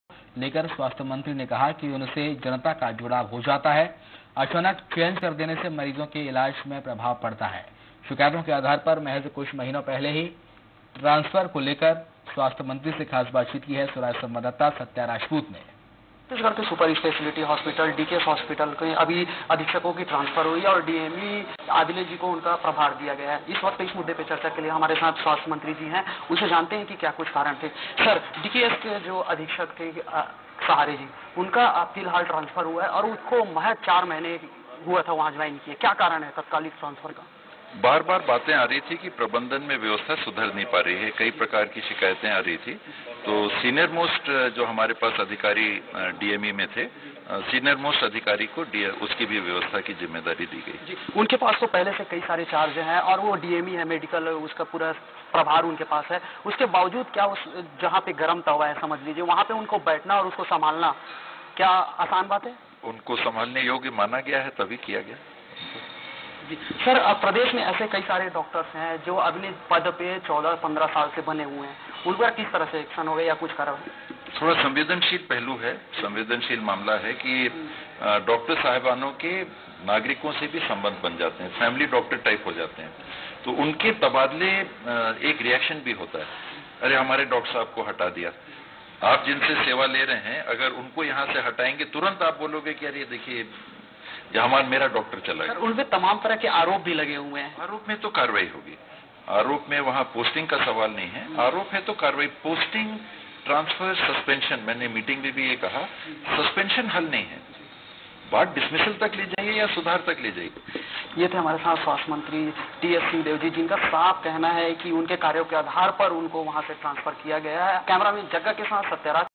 DKS हॉस्पिटल के अधीक्षक ट्रांसफर को लेकर स्वास्थ्य मंत्री से ख़ास बातचीत
DKS हॉस्पिटल के अधीक्षक ट्रांसफर को लेकर स्वास्थ्य मंत्री से ख़ास बातचीत की स्वराज संवाददाता सत्या राजपूत ने